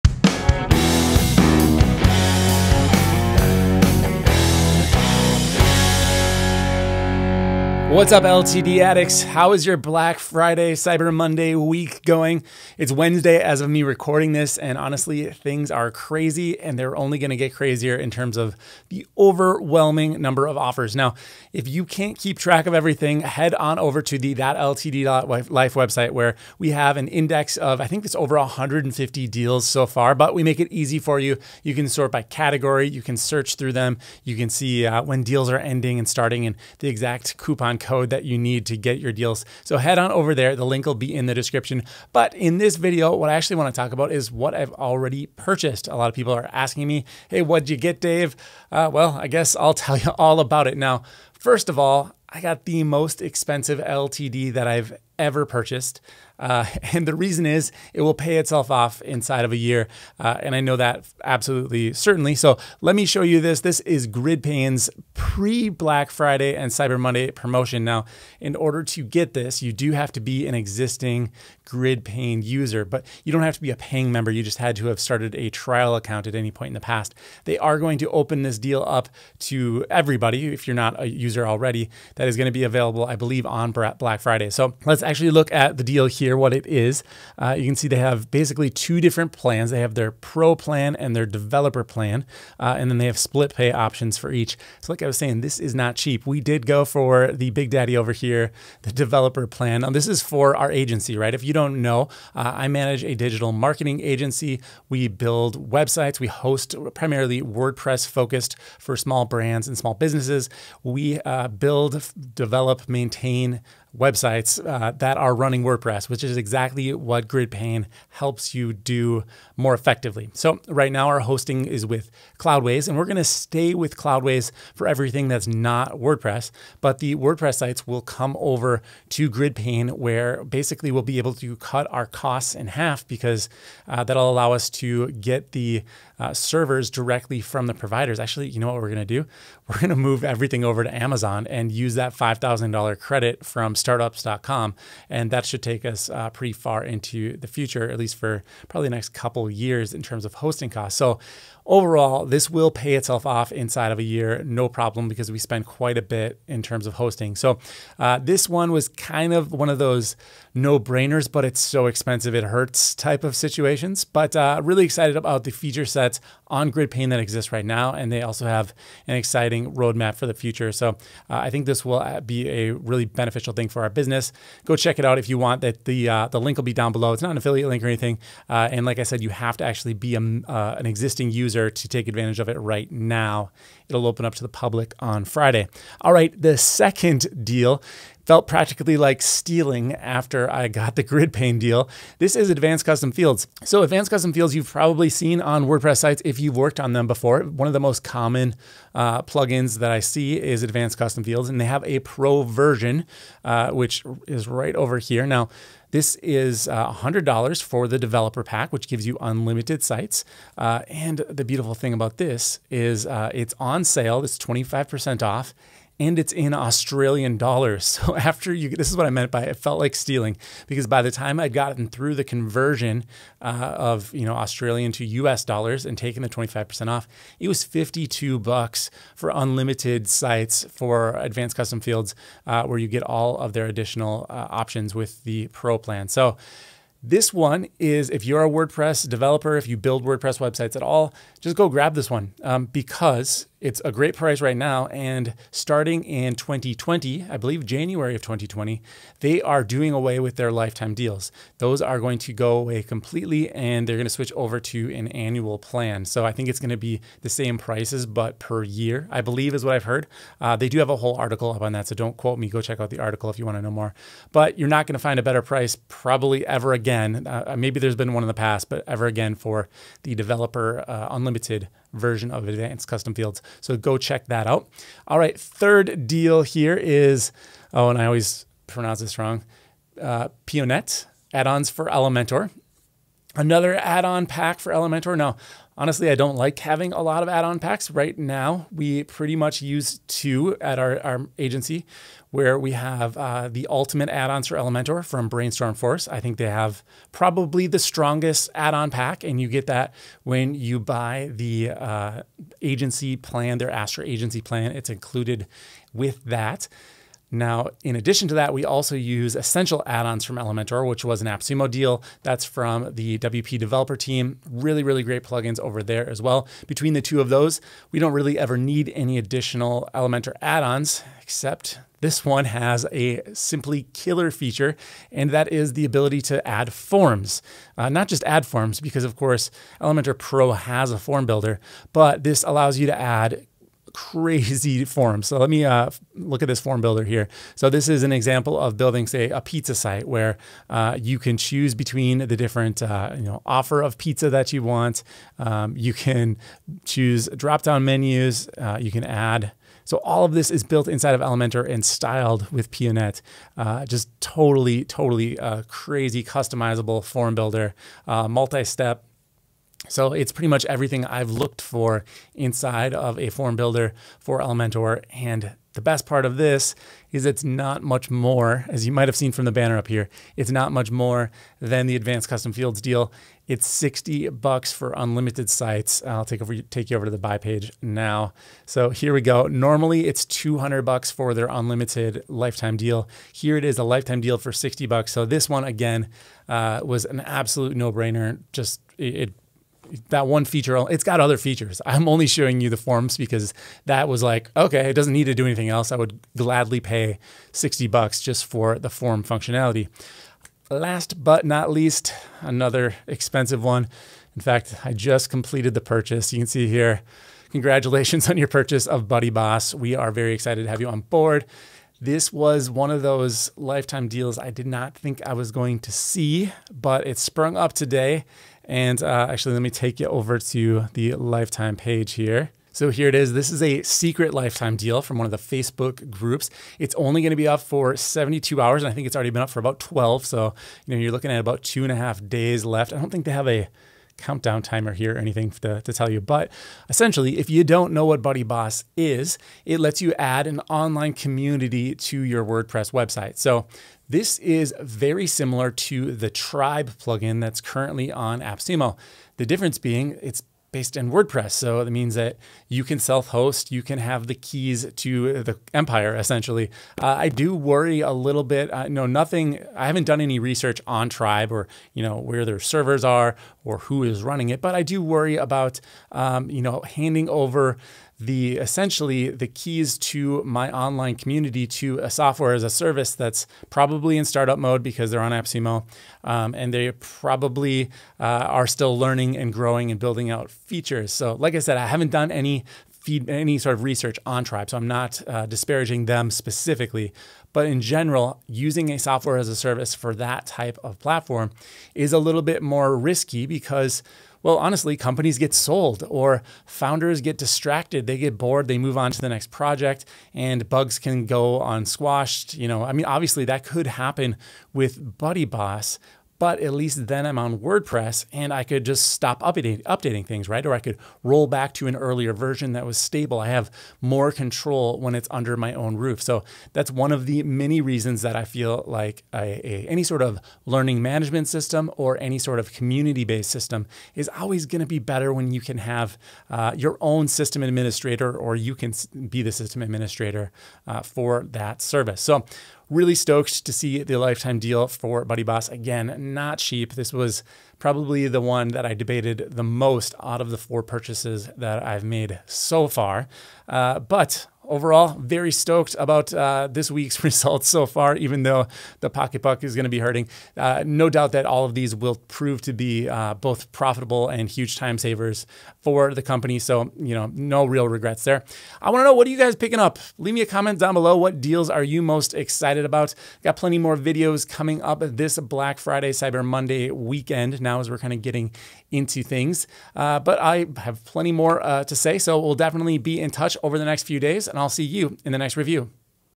My Most Expensive LTD EVER (What I Got This Black Friday)
What I Got This Black Friday!

Black Friday hasn’t even arrived yet but I’ve already purchased the most expensive LTD of my life!!!

In this video I’ll show you what’s in my shopping bag…

Spoiler: it’s two very high priced items and two items that feel like highway robbery.

Deals Mentioned in this video:
GridPane